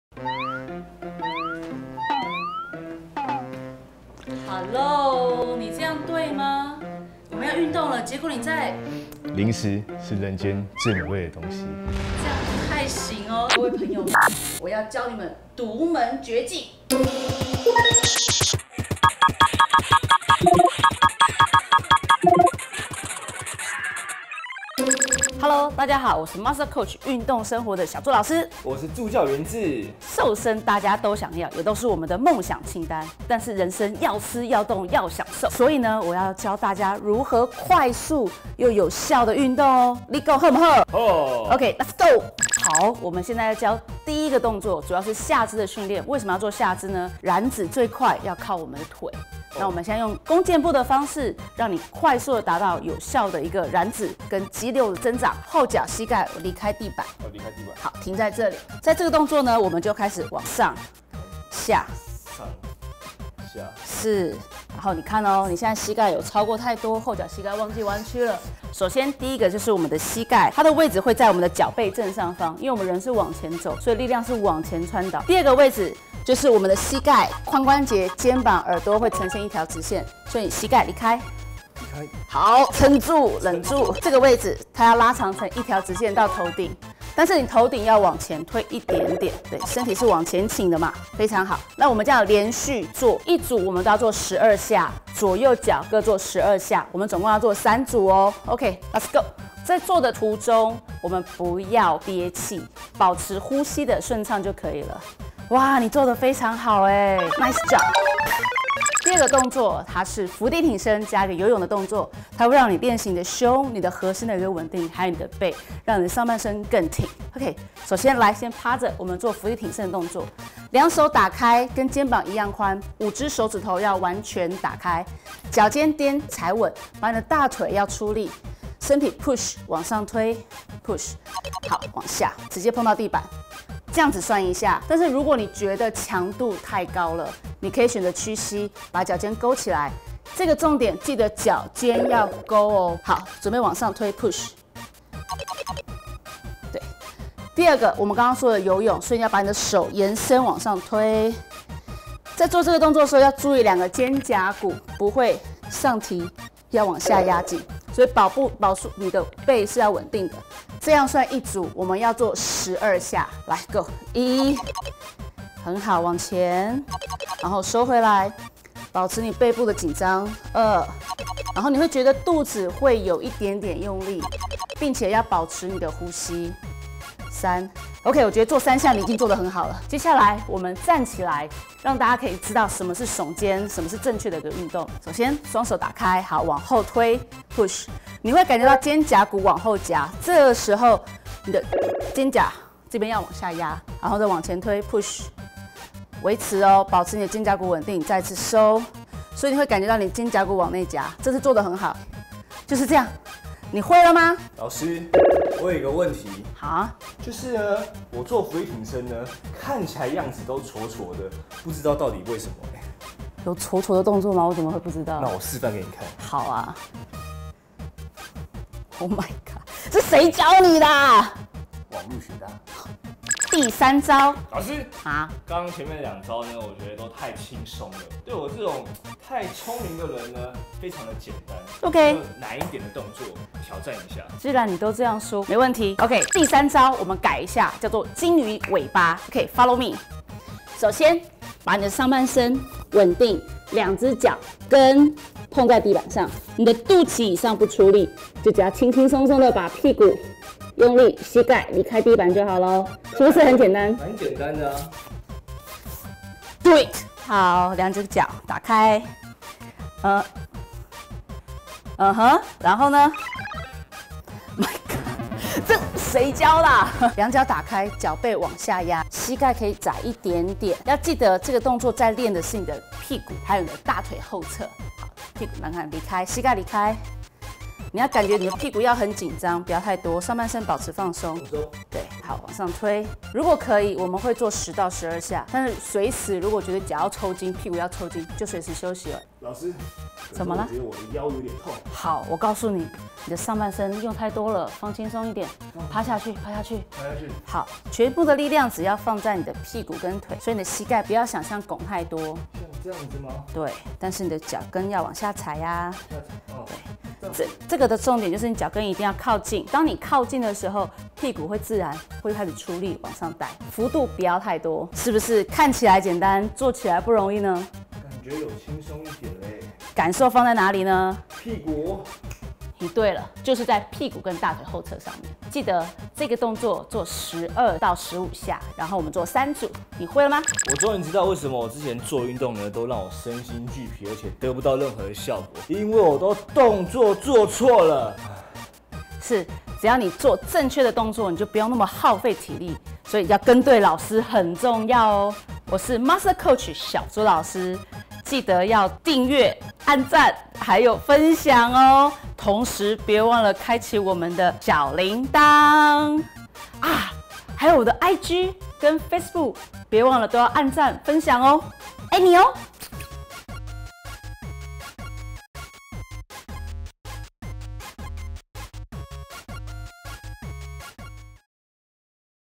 【60秒見效！在家就能做的「燃脂三招」 Mastercoach《EP1.運動生活體態篇》WebTVAsia職人系列】
小茱老師耕耘運動知識多年，她發現運動可以有更多科學的方式保護、強化、提升，這種時候，日常簡單的伸展、單純的循環運動，對於身體肌肉關節，也都會有極大的幫助。

本集影片內容總共有循序漸進的三項招式

1.弓箭步
2.伏地挺身加游泳
3.鯨魚尾巴

透過正確的姿勢，讓你的運動成效明顯進步，一起來試試看吧

9/2起雙週更 隔周三晚上六點， WebTVAsia Taiwan全新職人系列－MasterCoach。

運動傷害x運動恢復保健的專家小茱老師讓你生活更健康自在！